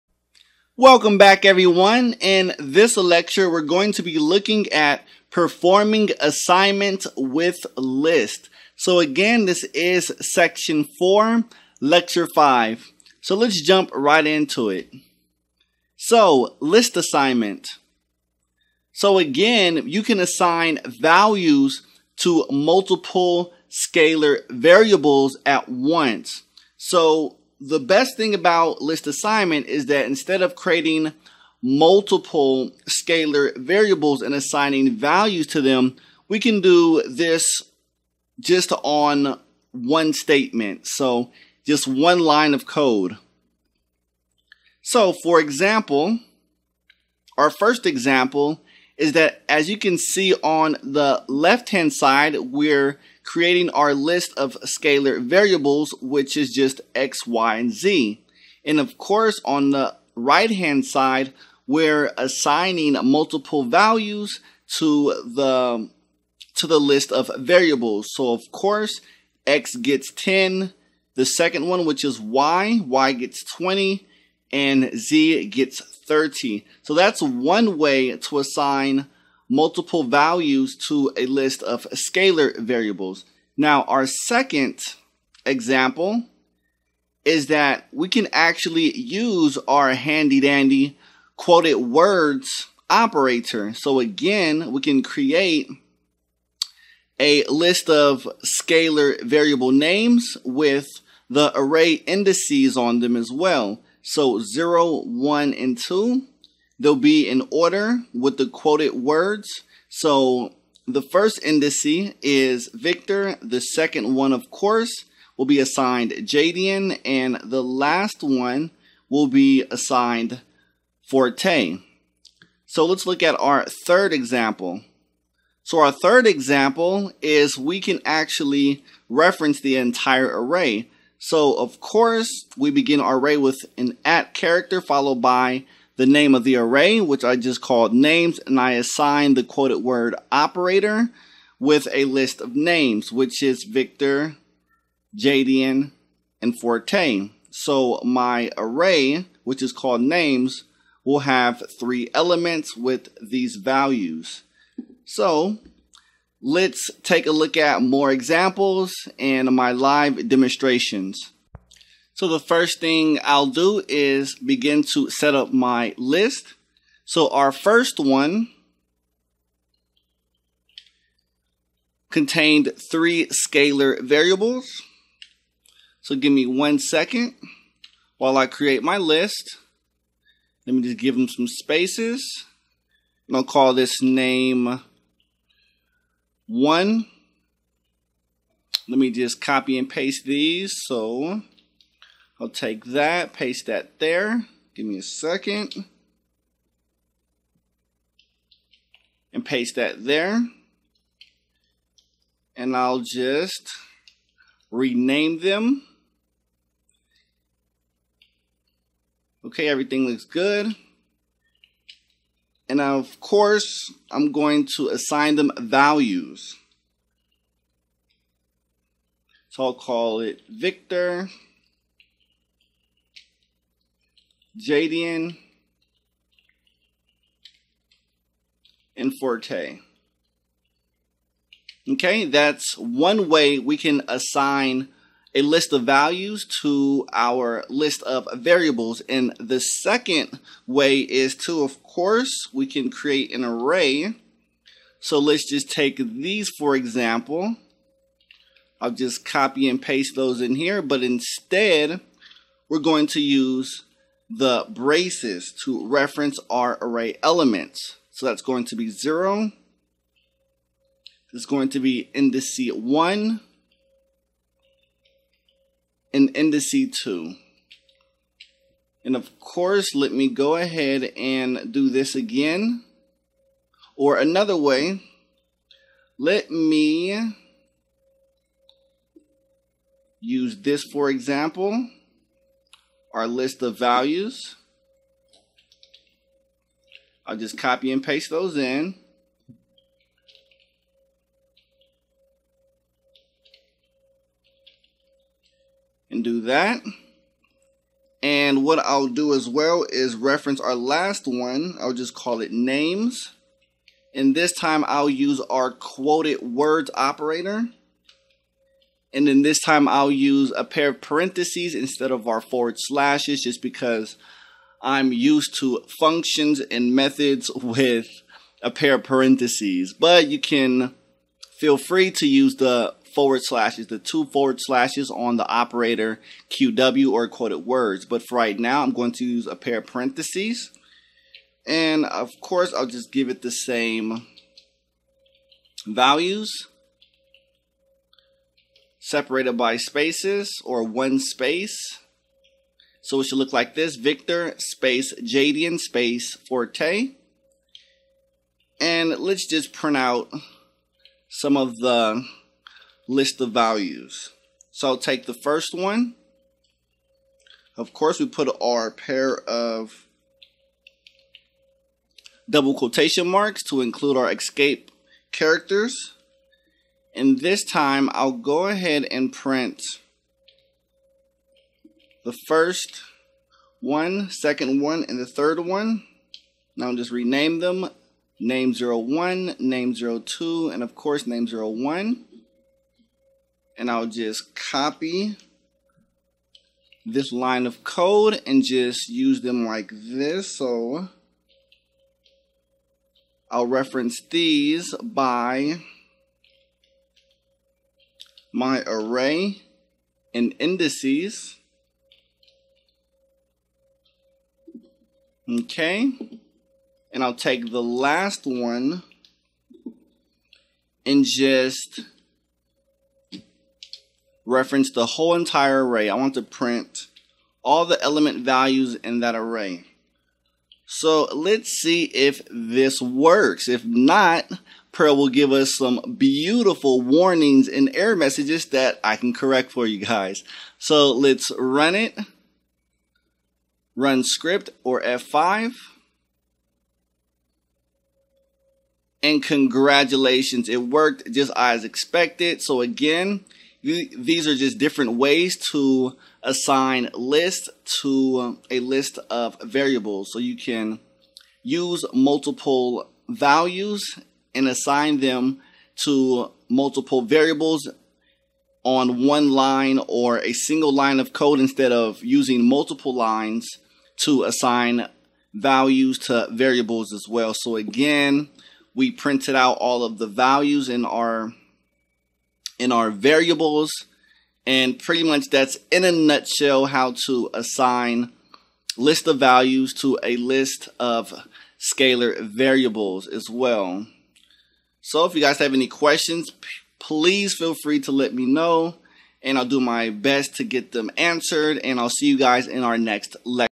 Perl Programming - Performing Assignment With Lists
Welcome back, everyone! In this lecture, we’re going to be looking at performing assignments with list. Again, this is section 4, lecture 5. Let’s jump right into it.
List assignment. You can assign values to multiple scalar variables at once. The best thing about list assignment is that instead of creating multiple scalar variables and assigning values to them, we can do this just on one statement, just one line of code.
Our first example is that as you can see on the left-hand side, we are creating our list of scalar variables which is just x y z. Of course, on the right-hand side, we’re assigning multiple values to the list of variables. Of course, x gets 10. The second one which is y gets 20. Z gets 30. That’s one way to assign multiple values to a list of scalar variables.
Our second example is that we can actually use our handy-dandy quoted words operator. We can create a list of scalar variable names with the array indices on them as well, so 0 1 2. They will be in order with the quoted words. The first indice is Victor. The second one of course will be assigned Jaddian. The last one will be assigned Forte.
Let’s look at our third example. Our third example is we can actually reference the entire array. Of course we begin our array with an @ character followed by the name of the array which I just called names and I assigned the quoted word operator with a list of names which is Victor Jaddian and Forte. My array which is called names will have three elements with these values.
Let’s take a look at more examples and my live demonstrations. The first thing I’ll do is begin to set up my list. Our first one contained three scalar variables. Give me one second while I create my list. Let me just give them some spaces. I’ll call this name01. Let me just copy and paste these. I’ll take that. Paste that there. I’ll just rename them. Everything looks good. Of course I’m going to assign them values. I’ll call it Victor Jaddian Forte. That’s one way we can assign a list of values to our list of variables.
The second way is to of course we can create an array. Let’s just take these for example. I’ll just copy and paste those in here. But instead, we’re going to use the braces to reference our array element. That’s going to be 0. It’s going to be indice 1 and indice 2. Let me go ahead and do this again or another way. Let me use this for example, our list of values. I’ll just copy and paste those in and do that. What I’ll do as well is reference our last one. I’ll just call it names. This time, I’ll use our quoted words operator. This time I’ll use a pair of parentheses instead of our forward slashes just because I’m used to functions and methods with a pair of parentheses. But you can feel free to use the two forward slashes on the operator qw or quoted words. But for right now, I’m going to use a pair of parentheses. Of course, I’ll just give it the same values separated by spaces or one space. It should look like this Victor Jaddian Forte. Let’s just print out some of the list of values. I’ll take the first one. Of course we put our pair of double quotation marks to include our escape characters. This time I’ll go ahead and print the first one, second one, and the third one. I’ll just rename them name01, name02 and name03. I’ll just copy this line of code and just use them like this. I’ll reference these by my array and indices. I’ll take the last one and just reference the whole entire array. I want to print all the element values in that array. Let’s see if this works. If not, Perl will give us some beautiful, wonderful warnings and error messages that I can correct for you guys. Let’s run it. Run Script or F5. Congratulations! It worked just as I expected.
Again, these are just different ways to assign lists to a list of variables. You can use multiple values and assign them to multiple variables on one line or a single line of code instead of using multiple lines to assign values to variables as well. Again, we printed out all of the values in our variables. Pretty much that’s in a nutshell how to assign list of values to a list of scalar variables as well. If you guys have any questions, please feel free to let me know and I’ll do my best to get them answered. I’ll see you guys in our next lecture.